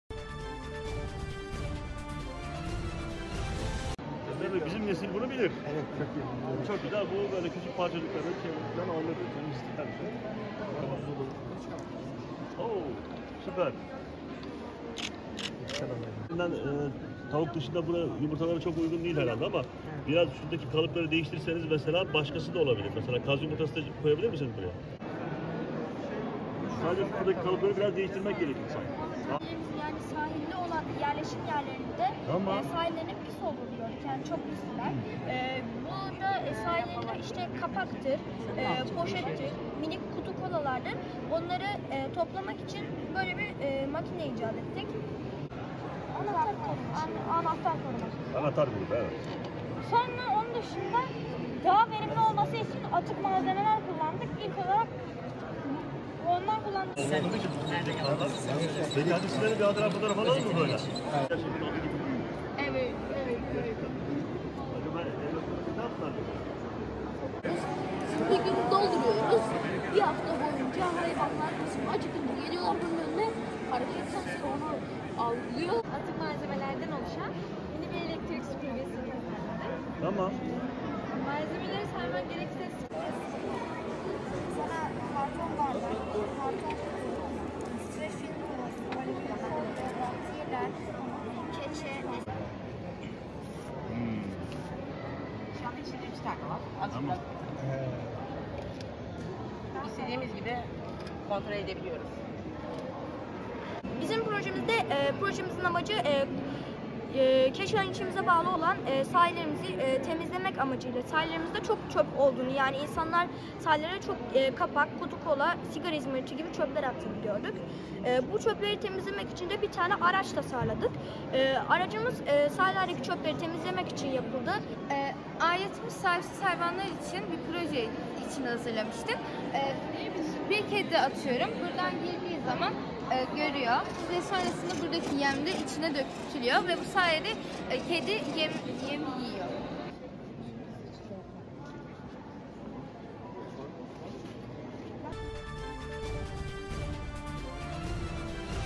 Öğrenciler çevreci robotları tanıttı
Edirne'de öğrenciler atık malzemelerden yaptıkları robotları tanıttı.
İl Milli Eğitim Müdürlüğünce Edirne Bahçeşehir Koleji Spor Salonu'nda düzenlenen "Robotlar Yarışıyor" adlı etkinlikte öğrenciler, tasarladıkları hazır ardiuno (interaktif projeler geliştirmek için tasarlanan, elektronik donanım ve yazılım temelli bir geliştirme platformu) kitleri ile hareket edebilen 36 robotu beğeniye sundu.